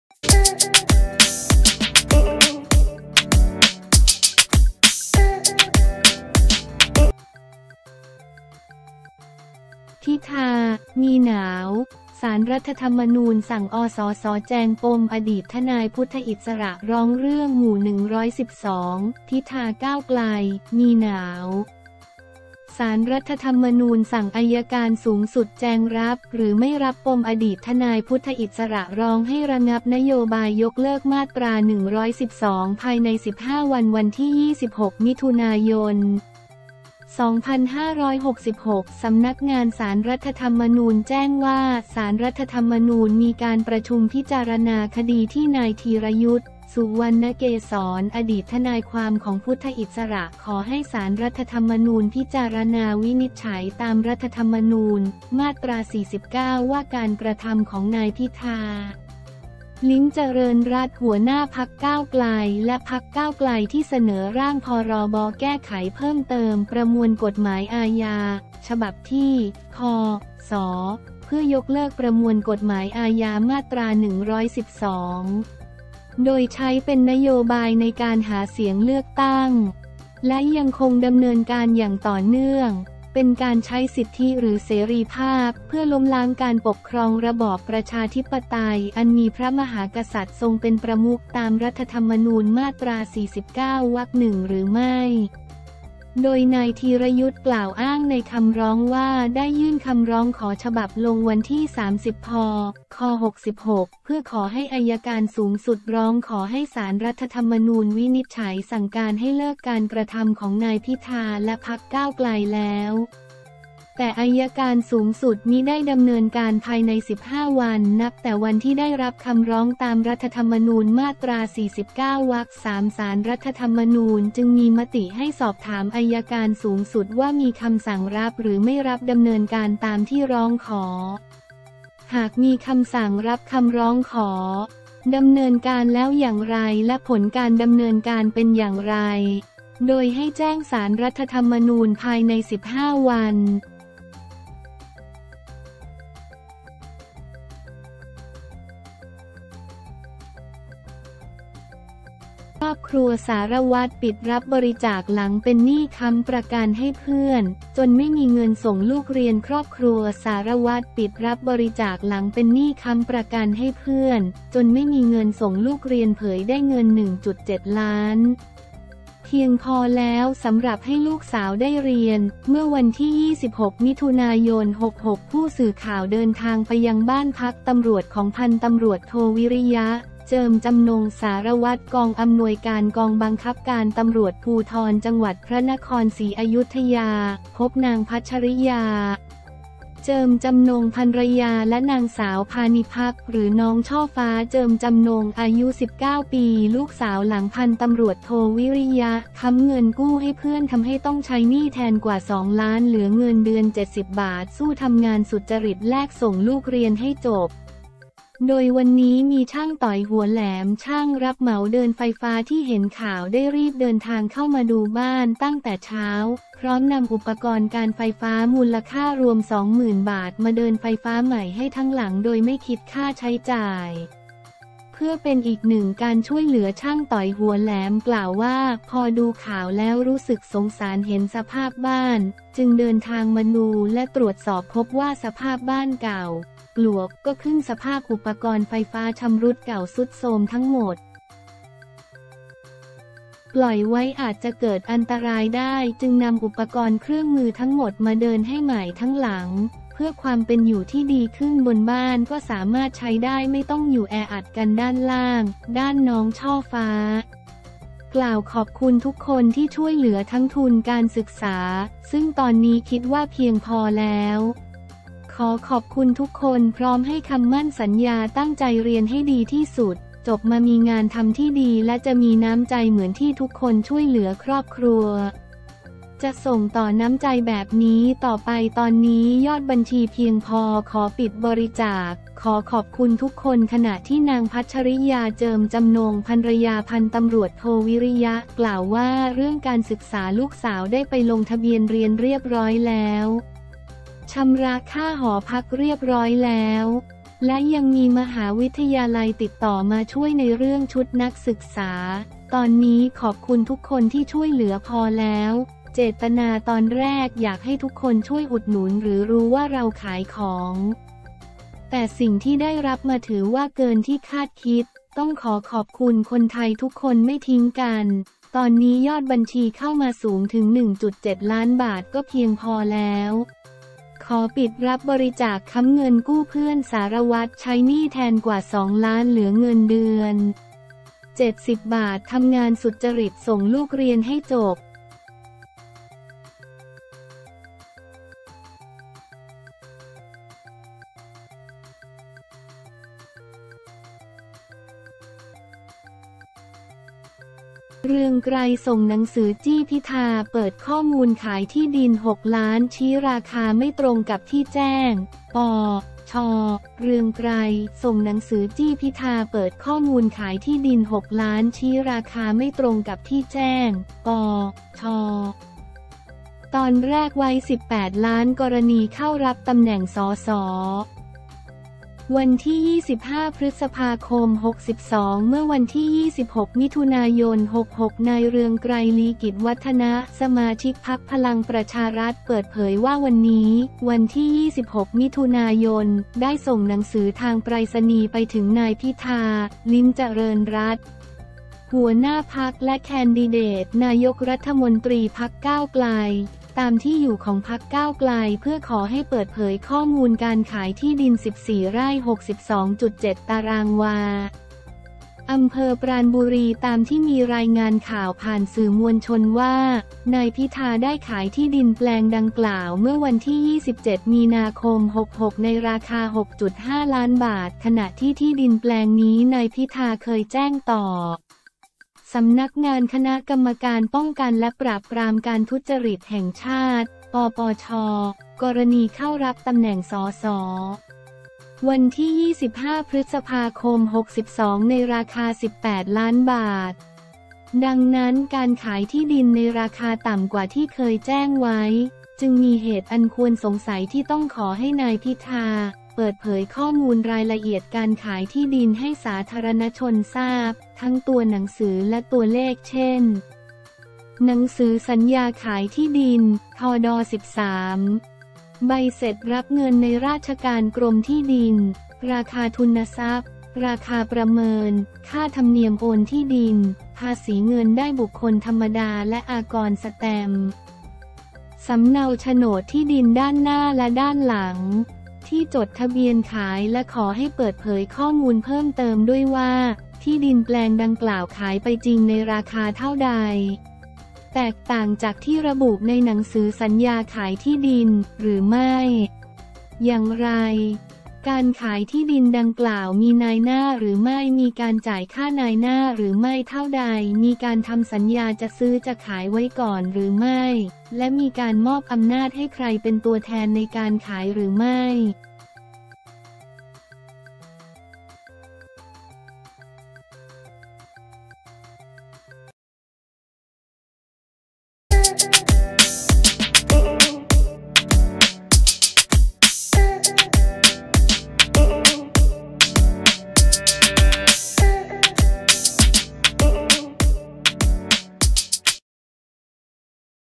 'พิธา' มีหนาว! ศาลรธน สั่งอสส แจงปมอดีตทนายพุทธอิสระ ร้องเรื่อง ม 112
‘พิธา-ก้าวไกล’ มีหนาว! ศาลรัฐธรรมนูญ สั่งอัยการสูงสุด แจงรับหรือไม่รับ ปมอดีตทนายพุทธอิสระ ร้องให้ระงับนโยบาย ยกเลิกมาตรา 112 ภายใน 15 วัน

วันที่ 26 มิ.ย. 2566 สำนักงานศาลรัฐธรรมนูญแจ้งว่า ศาลรัฐธรรมนูญมีการประชุมพิจารณาคดีที่นายธีรยุทธ สุวรรณเกษร อดีตทนายความของพุทธะอิสระ ขอให้ศาลรัฐธรรมนูญพิจารณาวินิจฉัยตามรัฐธรรมนูญ มาตรา 49 ว่าการกระทำของนายพิธา ลิ้มเจริญรัตน์ หัวหน้าพรรคก้าวไกล และพรรคก้าวไกลที่เสนอร่าง พ.ร.บ.แก้ไขเพิ่มเติมประมวลกฎหมายอาญา (ฉบับที่…) พ.ศ… เพื่อยกเลิกประมวลกฎหมายอาญามาตรา 112 โดยใช้เป็นนโยบายในการหาเสียงเลือกตั้ง และยังคงดำเนินการอย่างต่อเนื่อง เป็นการใช้สิทธิหรือเสรีภาพเพื่อล้มล้างการปกครองระบอบประชาธิปไตยอันมีพระมหากษัตริย์ทรงเป็นประมุขตามรัฐธรรมนูญมาตรา 49 วรรคหนึ่ง หรือไม่
โดยนายธีรยุทธกล่าวอ้างในคำร้องว่าได้ยื่นคำร้องขอฉบับลงวันที่ 30 พ.ค 66 เพื่อขอให้อัยการสูงสุดร้องขอให้ศาลรัฐธรรมนูญวินิจฉัยสั่งการให้เลิกการกระทำของนายพิธาและพรรคก้าวไกลแล้ว แต่อัยการสูงสุดมิได้ดำเนินการภายใน 15 วัน นับแต่วันที่ได้รับคำร้องตามรัฐธรรมนูญมาตรา 49 วรรคสาม

ศาลรัฐธรรมนูญจึงมีมติให้สอบถามอัยการสูงสุดว่ามีคำสั่งรับหรือไม่รับดำเนินการตามที่ร้องขอ หากมีคำสั่งรับคำร้องขอ ดำเนินการแล้วอย่างไร และผลการดำเนินการเป็นอย่างไร โดยให้แจ้งศาลรัฐธรรมนูญภายใน 15วัน